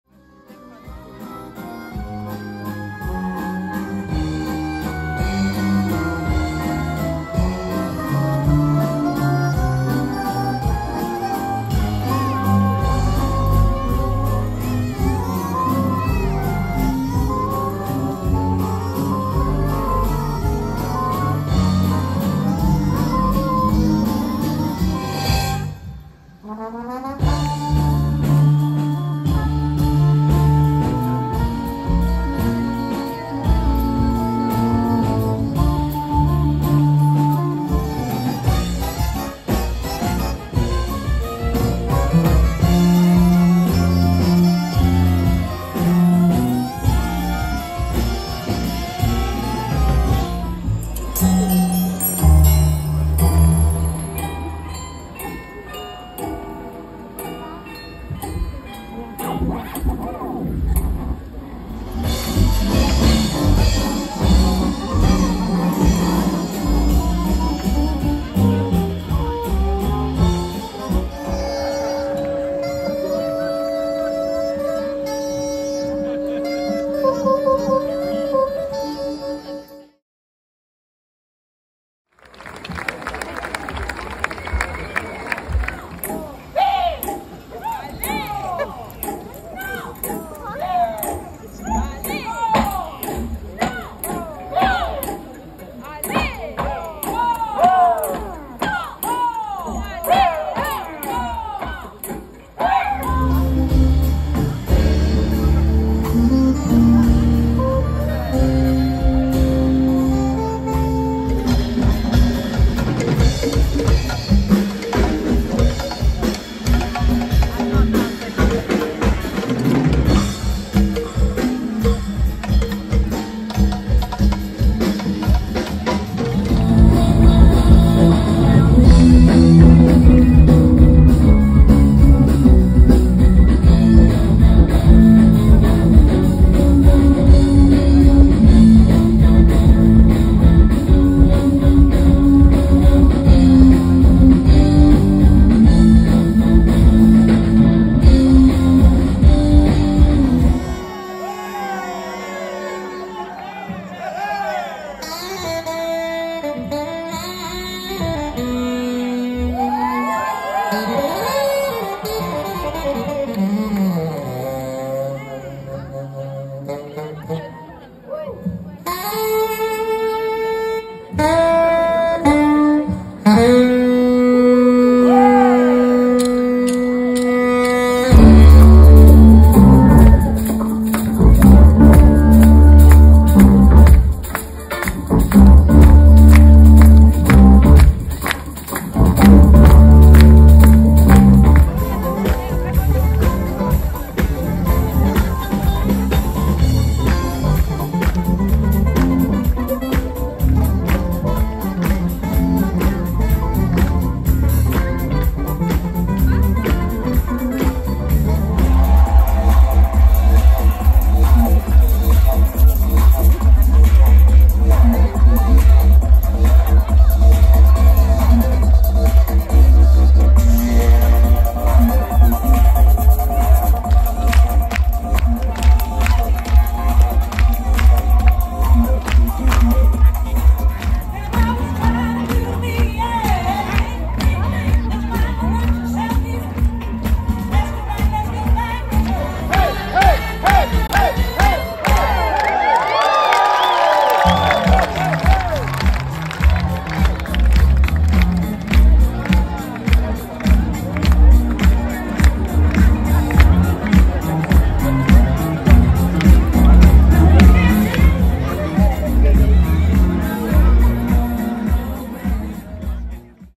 Epoustouflant Cristal Palace HD 1080p
Spectacle aérien grand format de la compagnie Transe Express,
art céleste, inventeur d'imaginaire, parades extraordinaires, 72 pays parcourus... un moment incroyable samedi 9 sept. à St Jean de Védas à côté de Montpellier  🤩